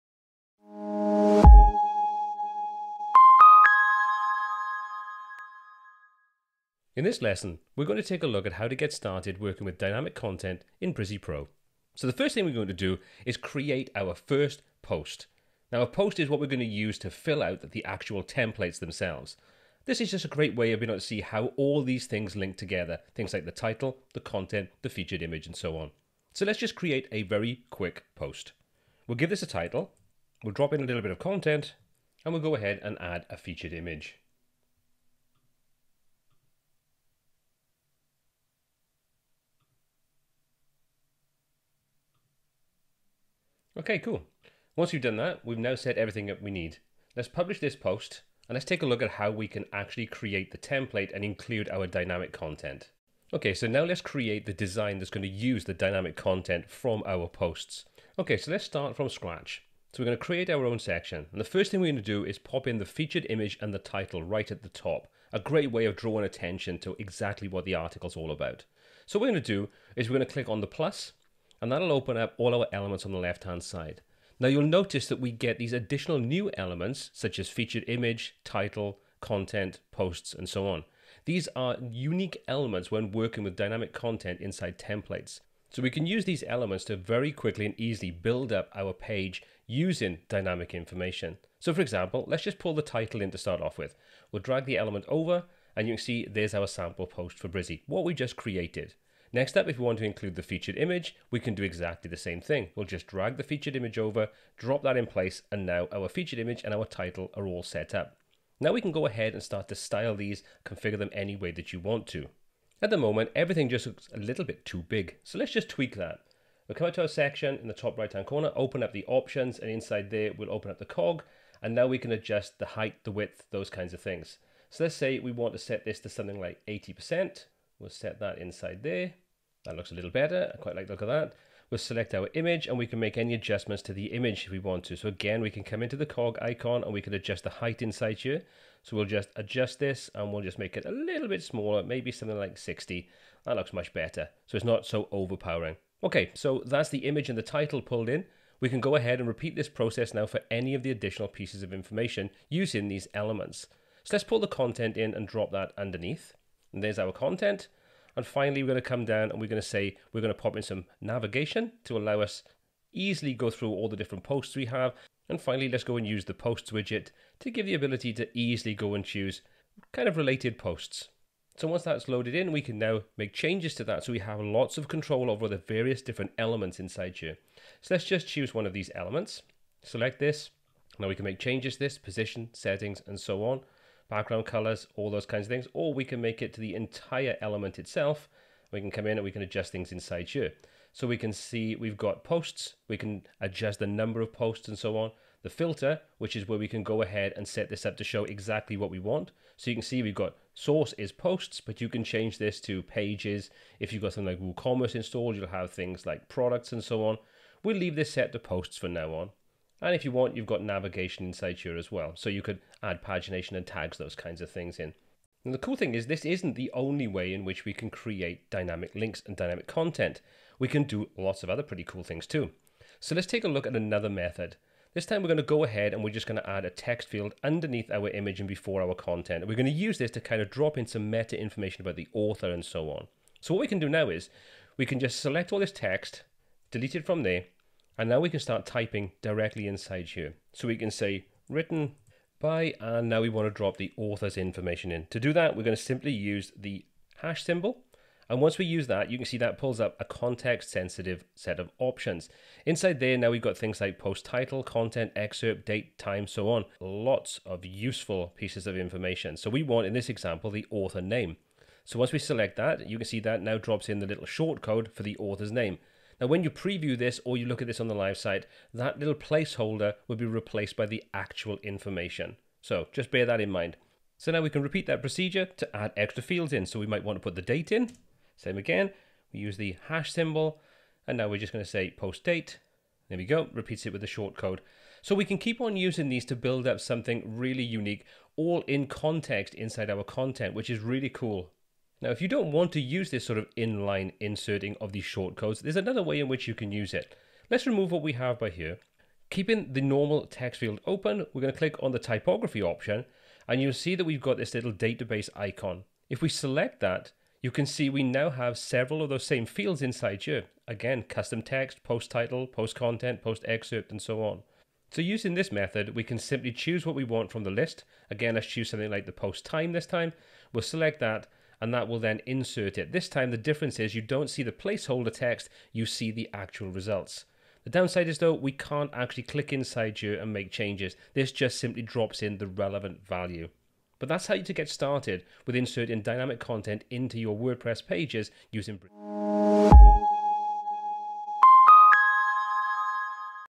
Harnessing the Potential of Dynamic Elements in Brizy | Lesson 18
👋 In this insightful lesson, join us as we delve into the exciting world of dynamic content in Brizy PRO for WordPress. Discover the power of dynamic elements and learn how to effortlessly incorporate dynamic content into your website. Unlock endless possibilities to showcase personalized and engaging content that dynamically responds to your visitors' interactions. Get ready to take your website to the next level with Brizy PRO's dynamic elements. Watch now and revolutionize the way you create and manage content!